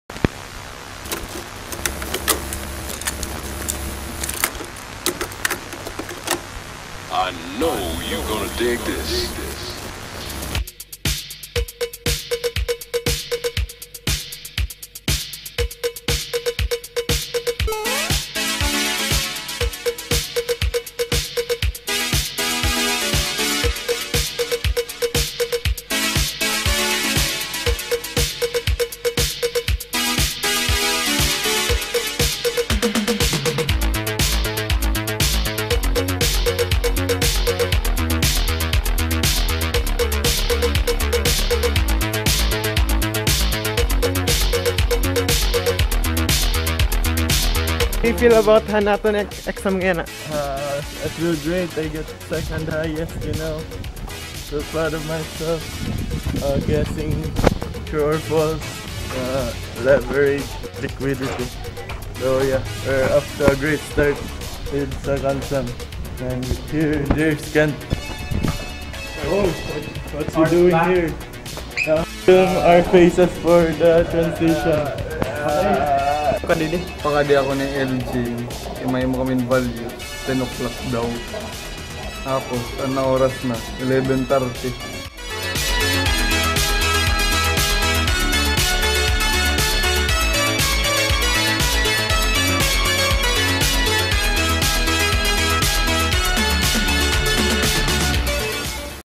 quiz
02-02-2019

Welcome to another short vlog documentary of how the day went. Join me as I make my way to school just to take a quiz.

99 days left

New videos everyday! (almost)